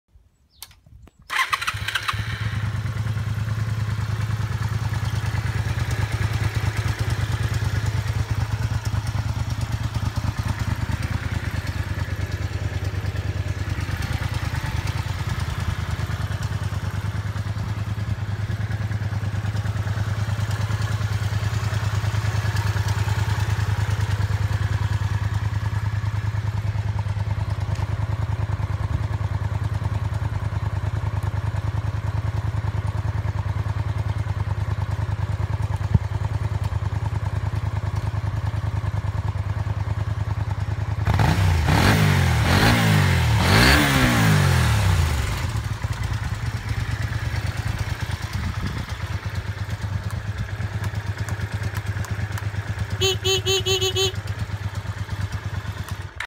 Видео запуска Honda VTR 250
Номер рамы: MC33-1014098
Видео запуска Honda VTR 250, более подробную информацию об этом мотоцикле вы можете найти на нашем сайте в разделе Каталог мототехники